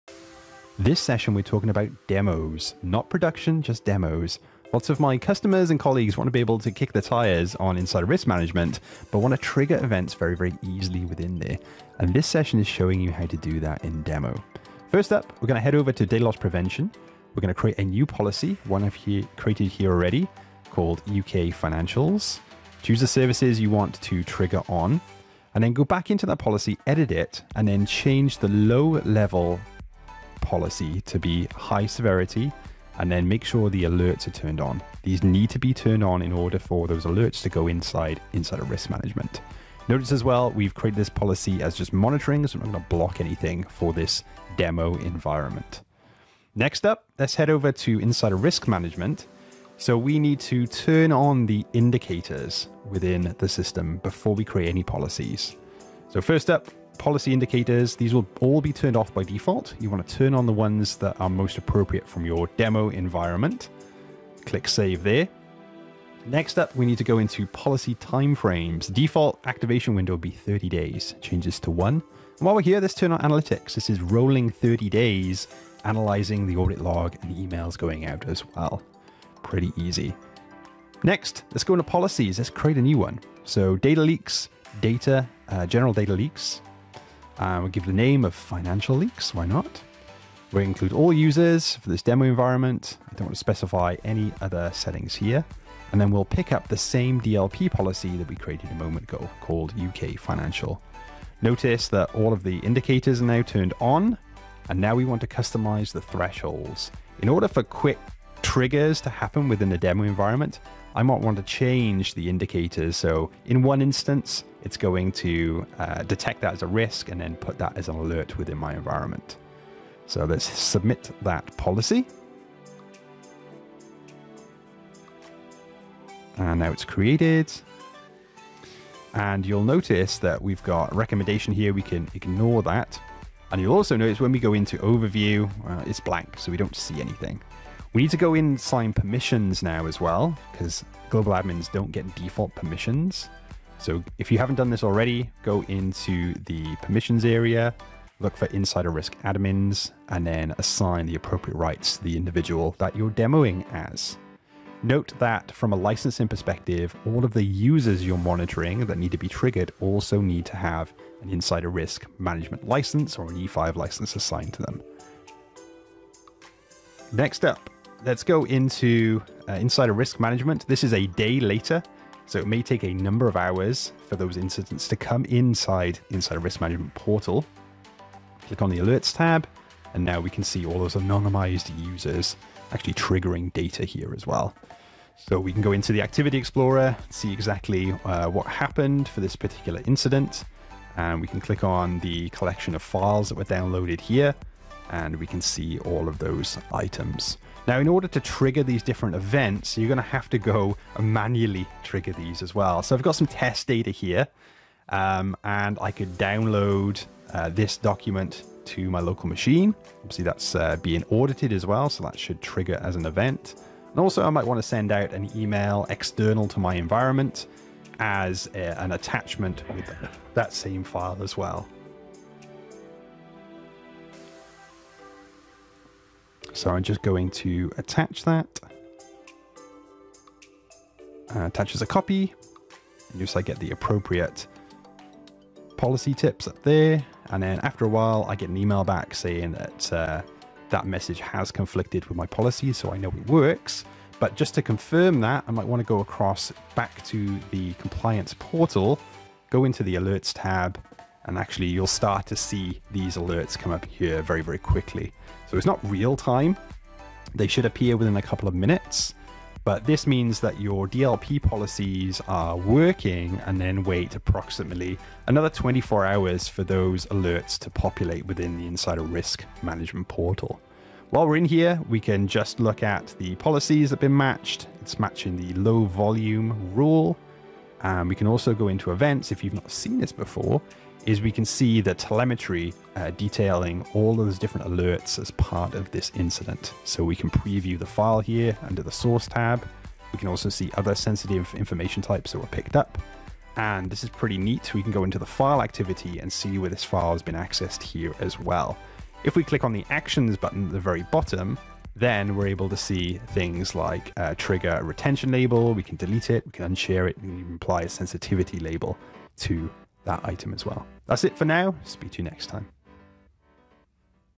Triggering Insider Risk for demo purposes....
Insider risk uses a number of metrics and behaviour insights before alerts are triggered, but what if you need to show/tell your organisation quickly and trigger items easily within a demo environment? Well, this short video will show you how. You will need need to wait 12-24 hours after the event was triggered to see alerts in IRM so be patient :D

Steps to setup and walkthrough to trigger:

1. Permissions:
Add IRM admin permissions to admin users.

2. DLP:
create Financial rule: monitor, policy tips.
change low rule to HIGH severity and alert admins
Set rule as No.1

3. IRM settings:
Enable indicators in settings
policy timeframe 1 day
intelligent detections - 1 event
alert volume - more alerts
turn on analytics

4. IRM Policy:
new policy "Financial Leaks" - trigger DLP rule "U.K Financial Data"
also assign activities - change to at least 1 event for each

5. Dummy data/trigger:
Download sensitive file from SharePoint